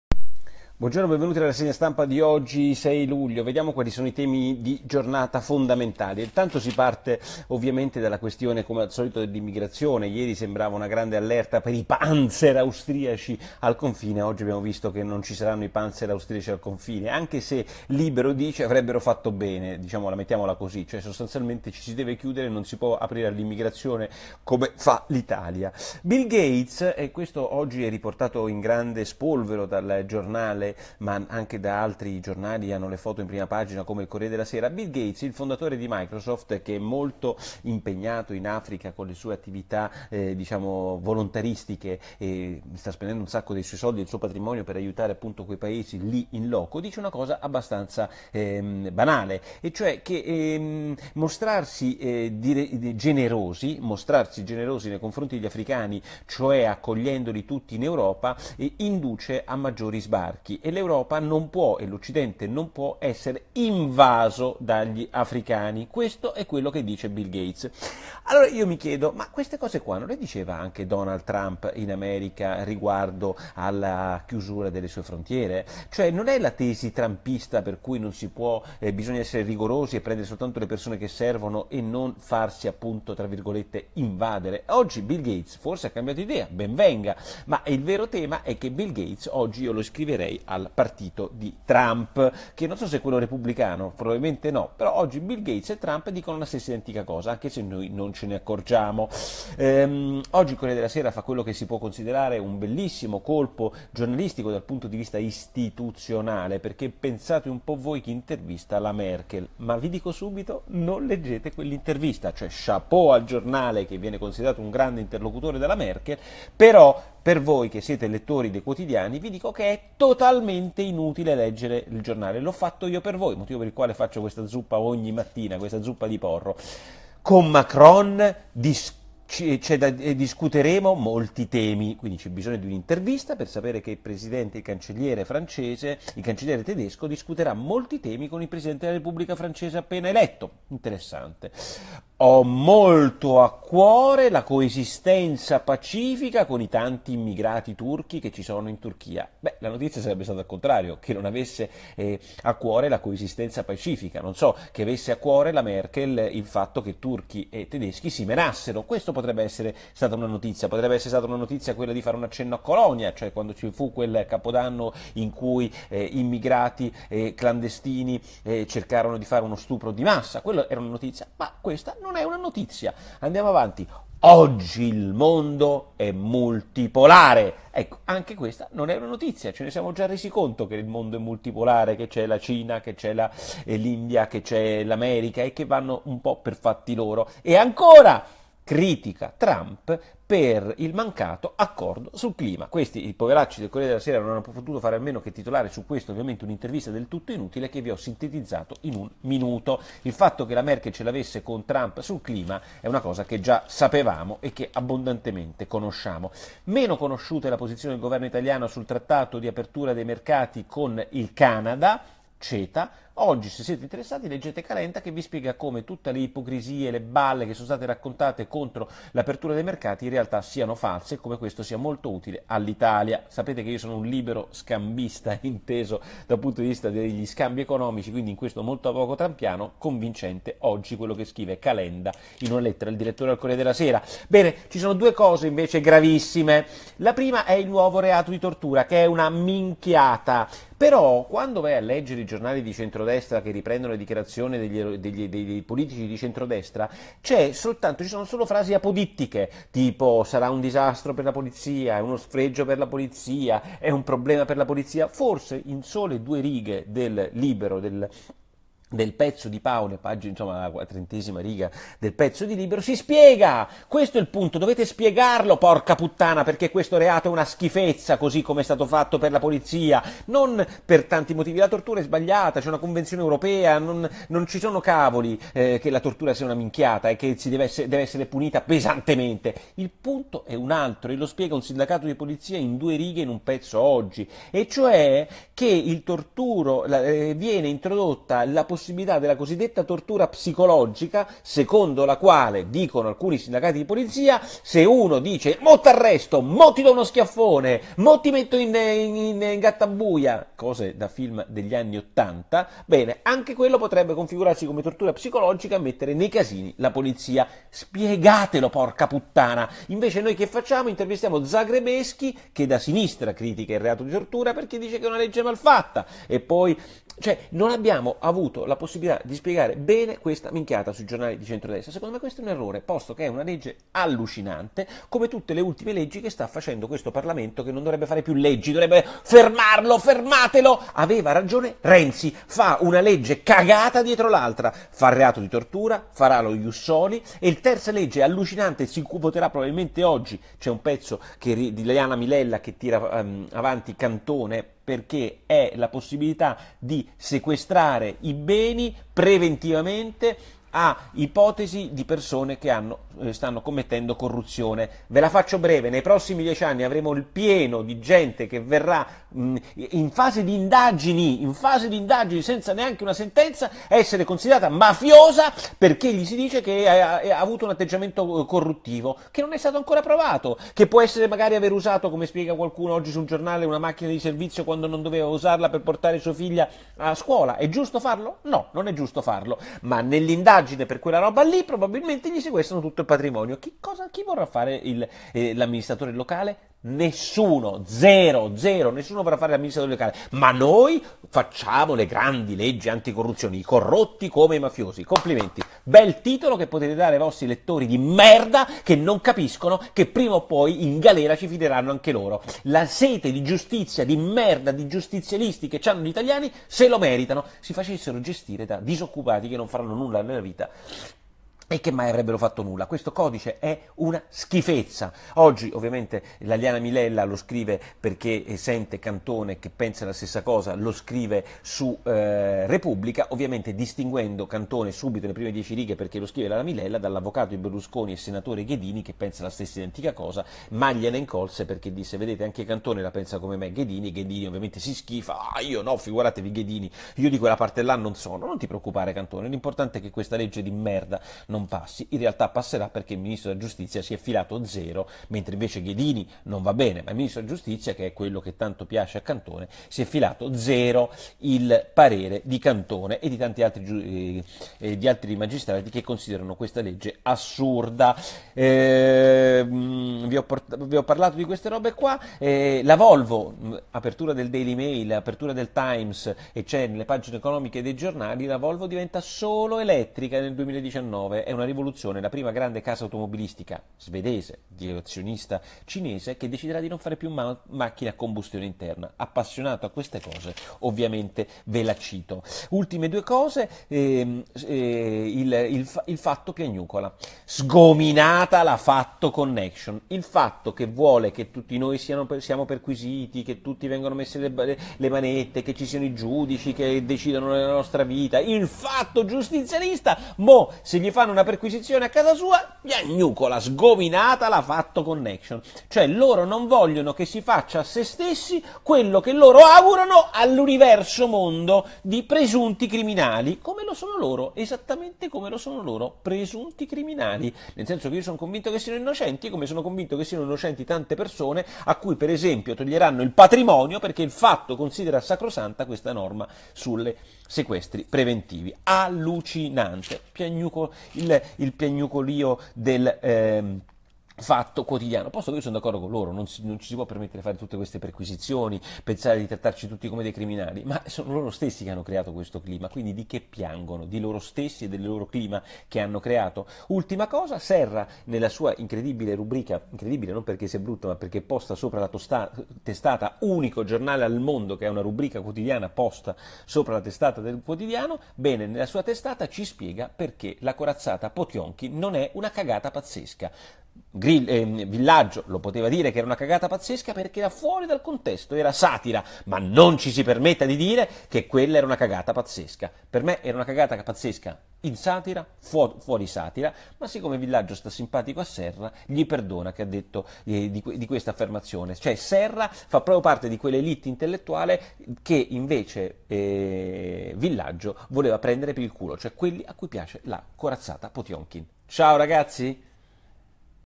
Bill Gates come Trump: l'Europa alzi i muri (6 luglio 2017)
00:00 Bill Gates diventa trumpiano: l'europa deve alzare i muri
01:40 Colpo del CorSera che intervista la Merkel. Ma lei non dice niente...
03:27 Il CETA e il libero scambio col Canada
03:55 Il nuovo reato di tortura è una minchiata. Ecco perché
05:40 Il codice anticorruzione e il sequestro preventivo figlia della sete giustizialista di merda
07:43 Dal 2019 la Volvo diventa elettrica
08:06 Allucinante, Il Fatto piagnucola
09:07 Corazzata Potemkin una cagata pazzesca? Per Serra no, per me sì